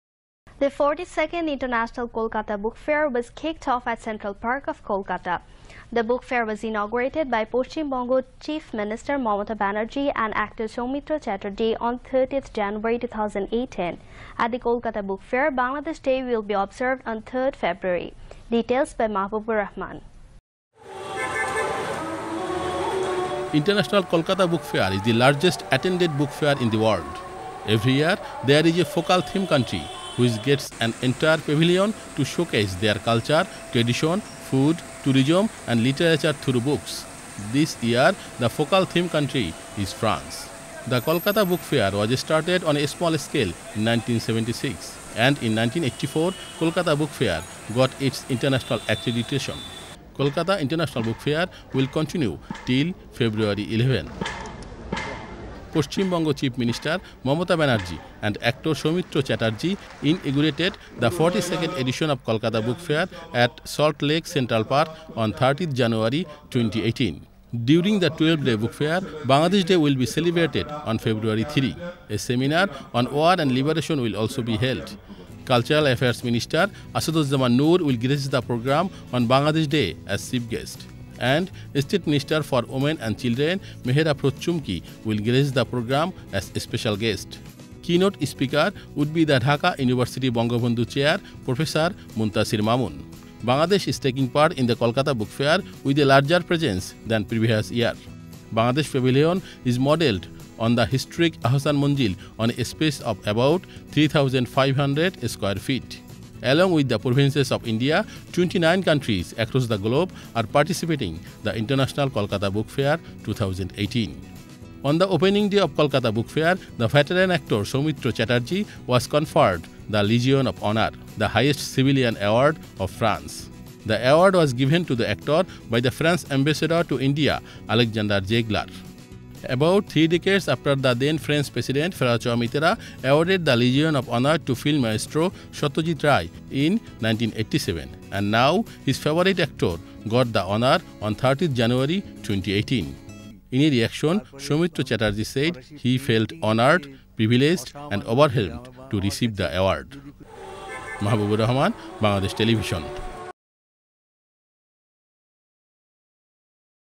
Kolkata International Book Fair - Mahbub Pkg in English BTV on air 31.01.18
42nd Kolkata International Book Fair was inaugurated on 30th January 2018. Th book fair at Central Park in Kolkata will be continued up to 11 February. Bangladesh Day will be observed on 3rd February. Here is the English report broadcast through BTV on 31.01.18